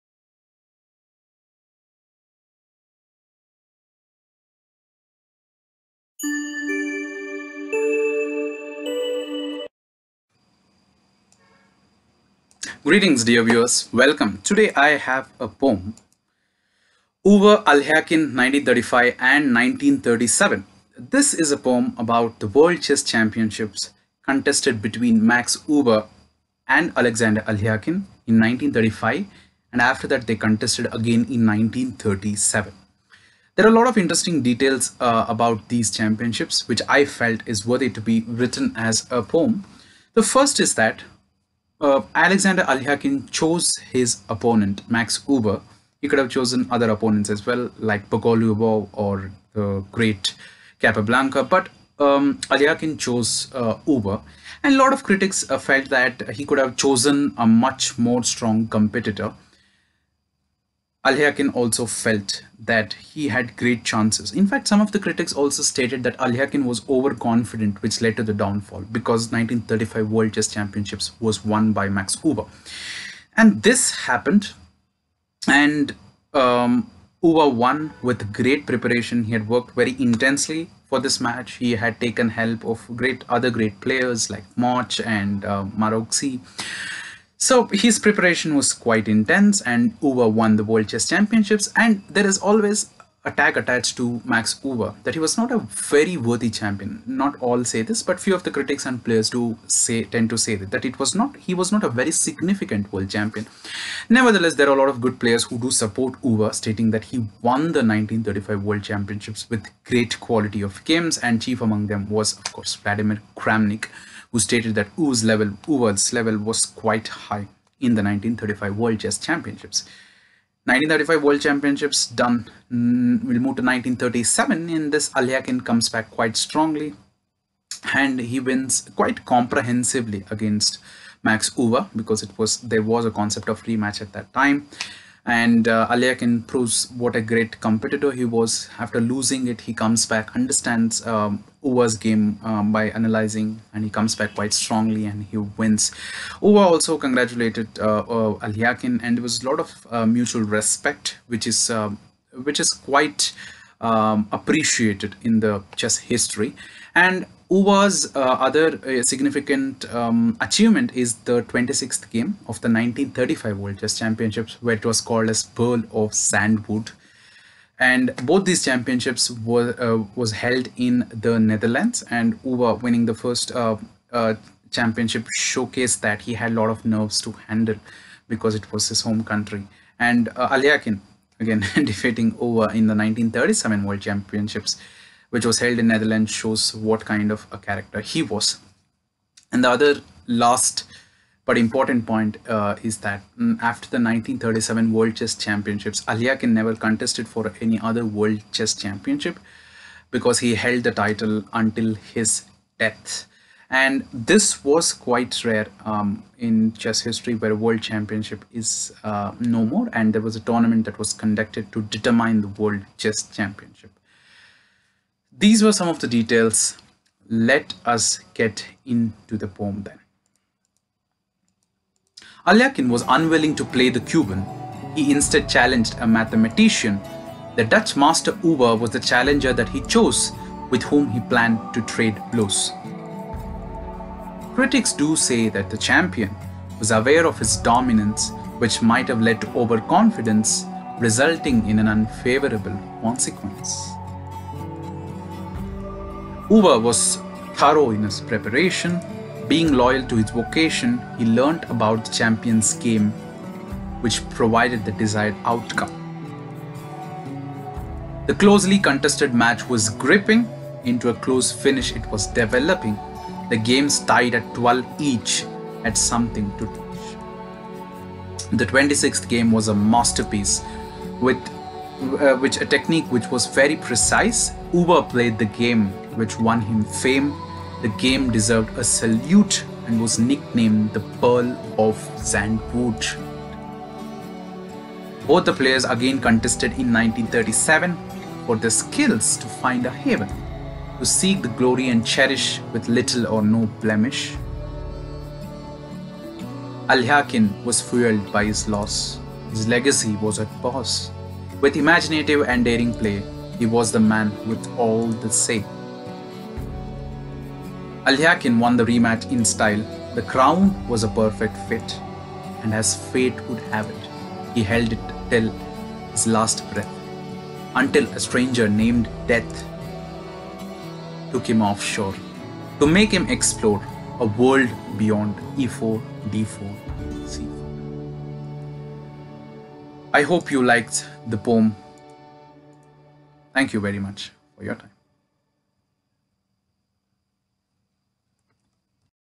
EUWE, ALEKHINE, 1935 & 1937!!
About the Poem:

Alekhine was unwilling to play Capablanca and Bogoljubov was no longer considered as a worthy challenger by many, Alekhine picked Max Euwe of Netherlands as the Challenger for the championships. Euwe was not a professional chess player and had a doctoral degree in Mathematics and later went on to work on programming as well!

The condition was the best of 30 games and 6 wins and Max Euwe emerged as the champion surprising many since he was not considered as the worthy contender for Alekhine.

Few of the analysts say that Alekhine’s complacence may had some impact in the match and there was a chance that he might have underestimated his opponent

The match was even after 24 games and in game 26, Euwe played a masterpiece which was named as ‘ The pearl of Zandvoort’!
Max Euwe had worked extremely hard for the match by working on his openings taking help from the likes of Becker, Kmoch and Maroczy since he carefully analyzed Alekhine games and prepared according to his strengths and weakness and rightly deserved the championship

Alekhine and Euwe again contested in 1937 and Alekhine was intent on making a statement since he had lost the previous match and he won the championship and many including Alekhine felt that the match was like exhibition. This proved how strongly Alekhine had prepared for the match
Both the players won the peoples hearts by showing great sportsmanship by heaping praise on each other’s efforts thereby setting an example for the future champions to come

Alekhine was unwilling to the play the Cuban,
He instead challenged a mathematician,
The Dutch master Euwe was the challenger that he chose,
With whom he planned to trade blows!

Critics do say that the champion,
Was aware of his dominance
Which might have led to overconfidence,
Resulting in an unfavourable consequence!

Euwe was thorough in his preparation,
Being loyal to his vocation,
He learnt about the champion’s game,
Which provided the desired outcome

The closely contested match was gripping,
Into a close finish, it was developing,
The games tied at 12 each(after 24 games),
Had something to teach!

The twenty sixth game was a masterpiece,
Which a technique which was very precise,
Euwe played the game ,
Which won him fame,
The game  deserved a salute,
And was nicknamed: ‘The pearl of Zandvoort!’

The challenger(Euwe) edged the champion,
To win the crown,
He did answer his critics,
That chess was in par with his mathematics!

Both the players again contested in 1937,
For their skills to find a haven,
To seek the glory and cherish,
With little or no blemish

Alekhine was fueled by his loss,
His legacy was at pause,
With imaginative and daring play,
He was the man with all the say!

Alekhine won the rematch in style,
The crown was a perfect fit
And As fate would have it,
He held it till until his last breath,
Until a stranger named ‘Death’!
 Took him offshore,
To make him explore,
A world beyond e4, d4, c4… .!

- Chesspoet64

Picture Source:
Chess.com
Chessgames.com